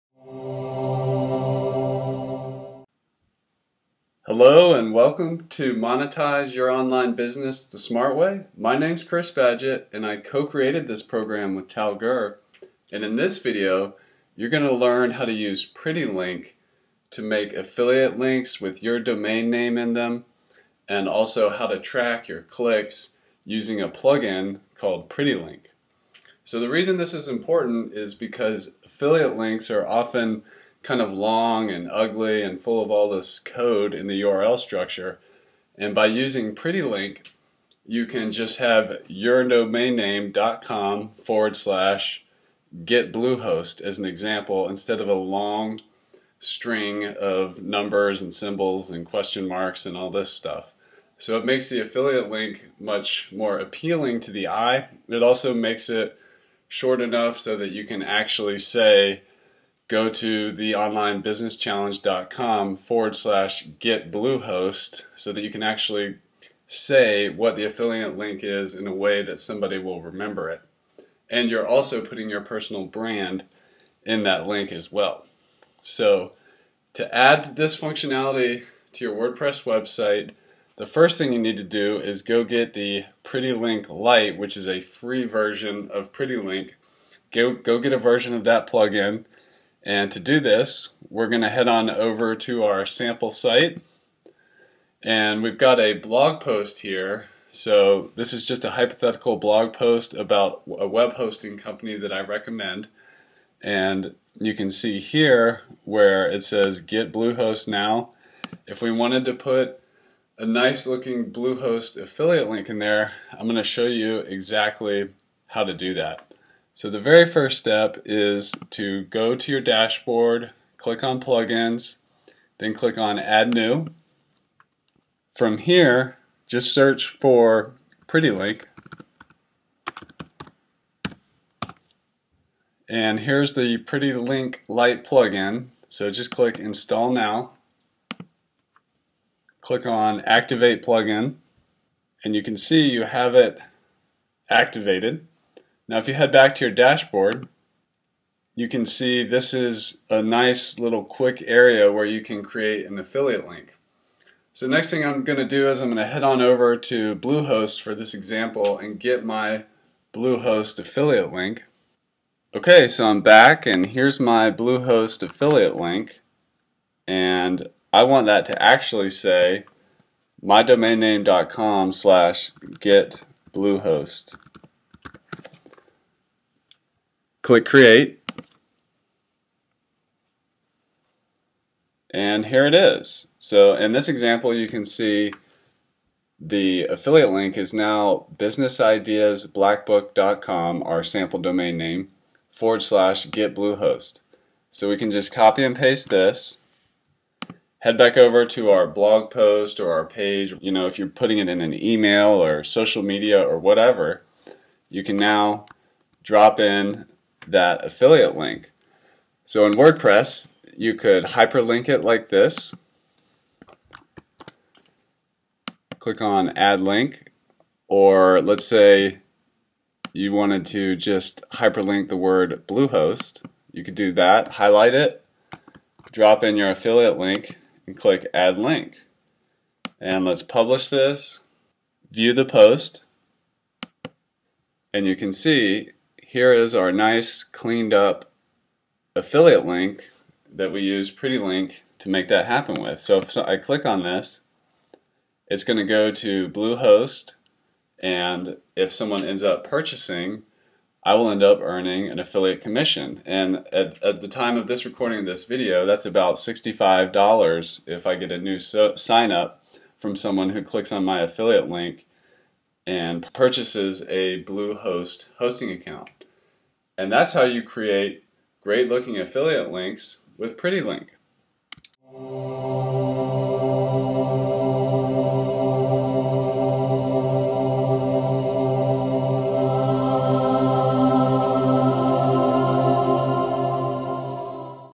How to Use Pretty Link to Create and Track Affiliate Links
Understanding and working with pretty link lite WordPress plugin for affiliate marketing. This is part of The Monetize Your Online Business the Smart Way training program.

There are 3 keys to becoming a successful affiliate marketer:
1. Picking the RIGHT products
2. Building trust and credibility
3. Driving the RIGHT traffic.

The first key is super important. You want to make sure that whichever product you're going to promote has to be 1. extremely useful to your audience or community, and 2. highly recommended by credible sources and fellow experts.